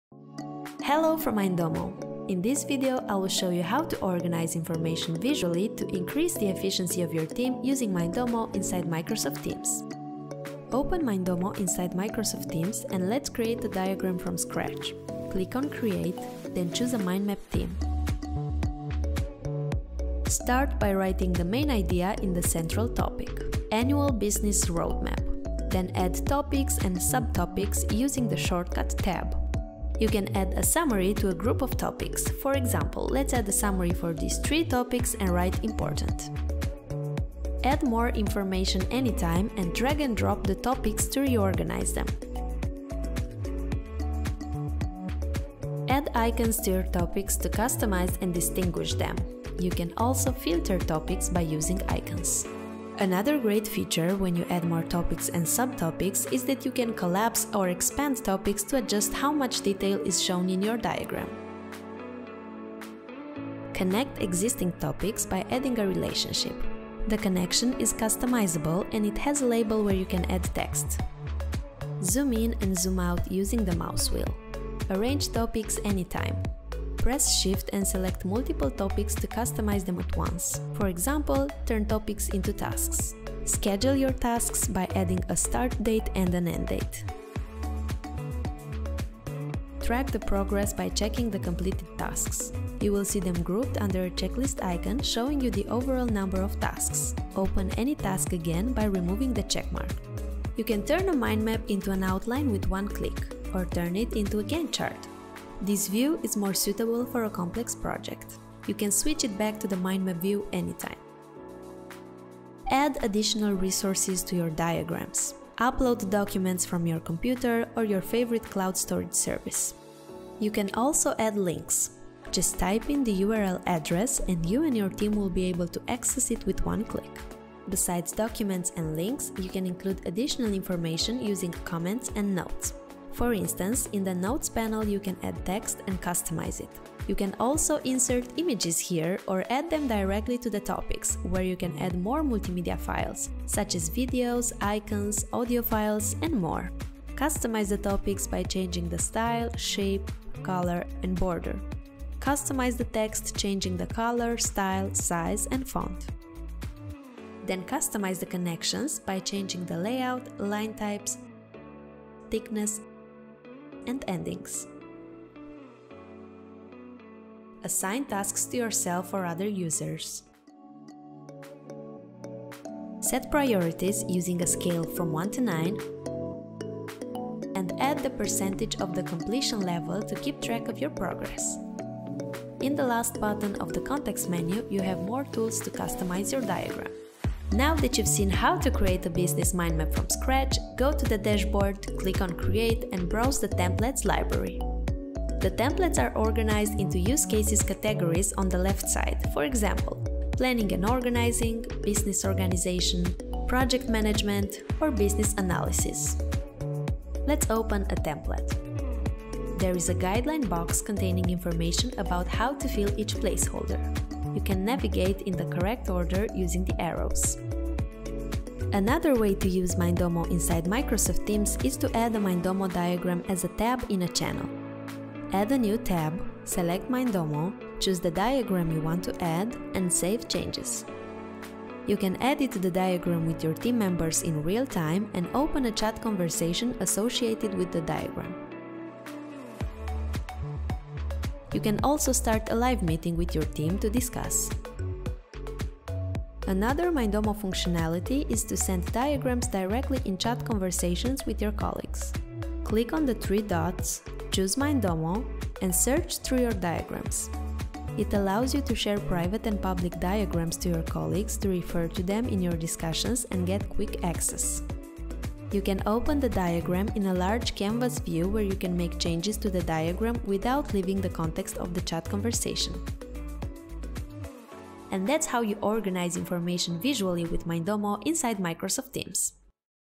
Mindomo - Microsoft Teams demo
This is a demonstration of how to use Mindomo inside Microsoft Teams.

Keep it smart, simple, and creative!